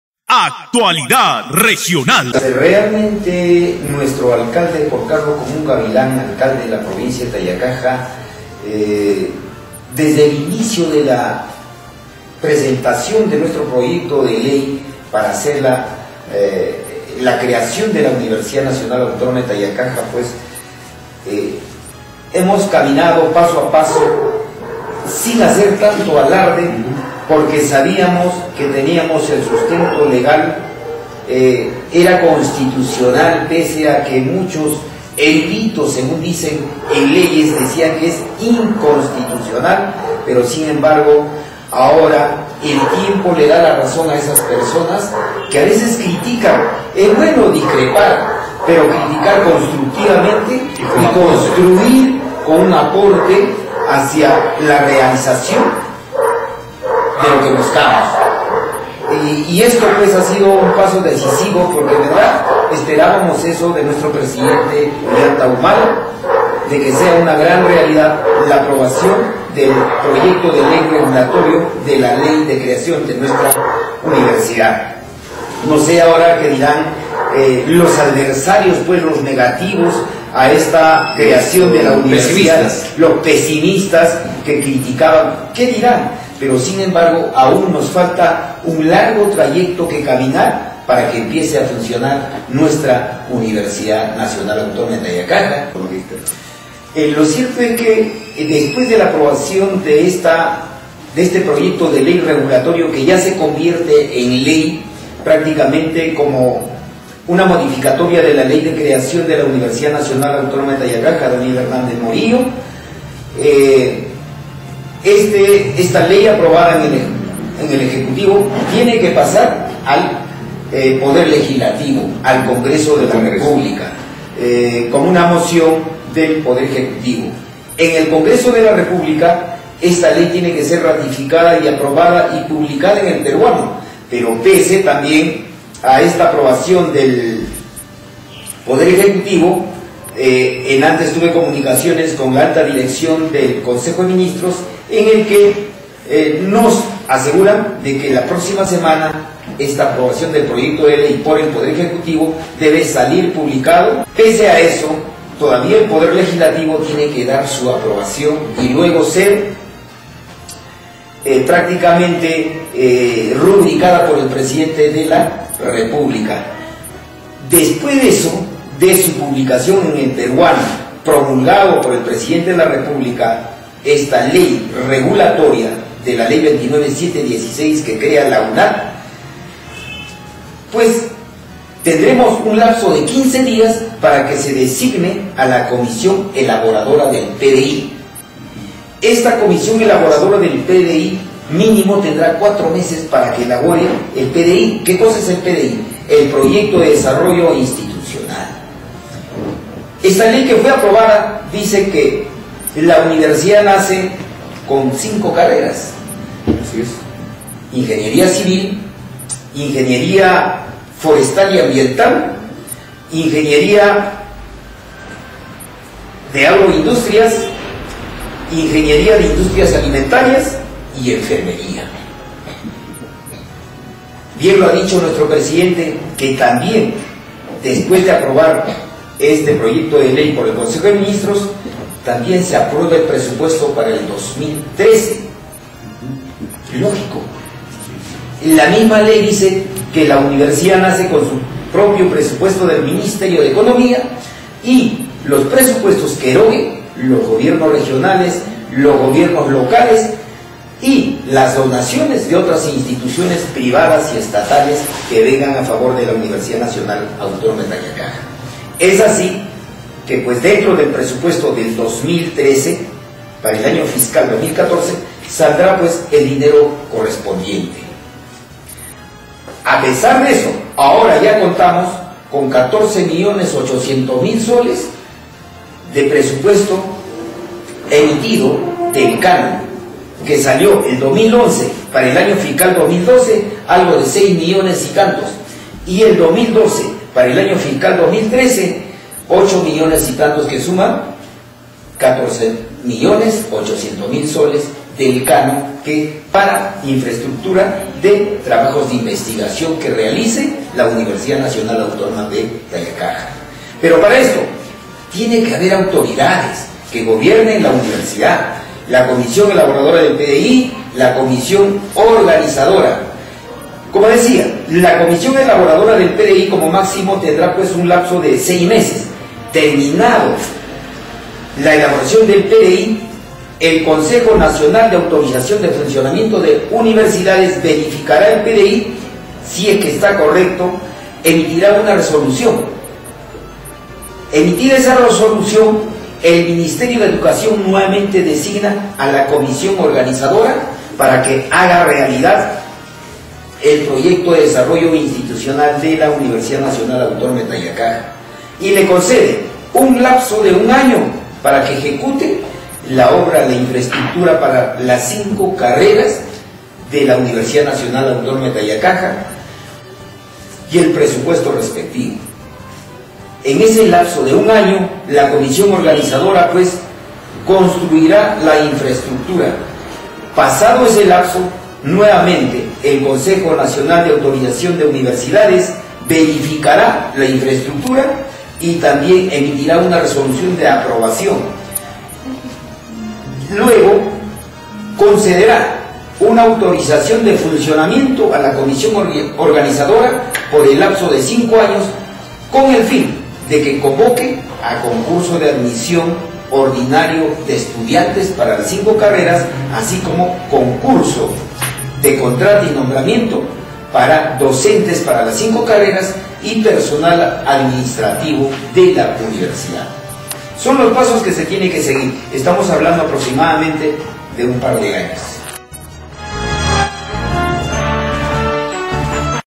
LA VERDAD DEL FUTURO DE LA UNIVERSIDAD DE TAYACAJA-UNAT.
Programa Informativo. "ACTUALIDAD REGIONAL", reproduce entrevista al Ing. Ángel Almidón Elescano. donde explica sobre los próximos pasos a seguir en el funcionamiento de la UNAT.